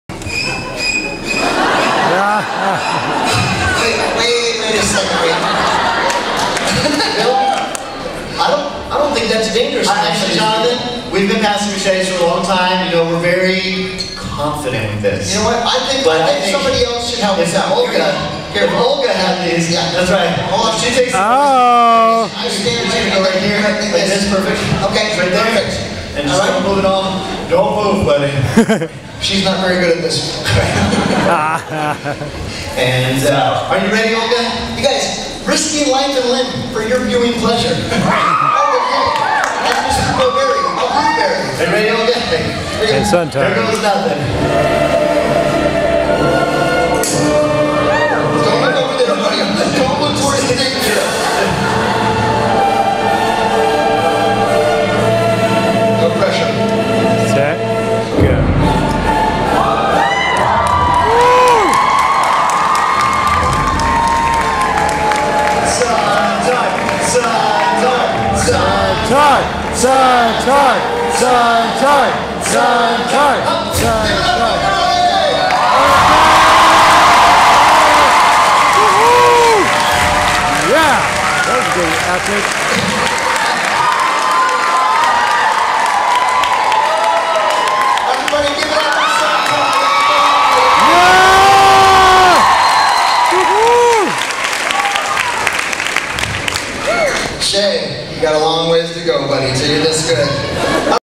Epic Knife Throwing with Olga Kay, RootBerry and Sontard
Olga Kay and a member RootBerry are throwing knives in a epic way.  this video is also starring Sontard from the Shaytards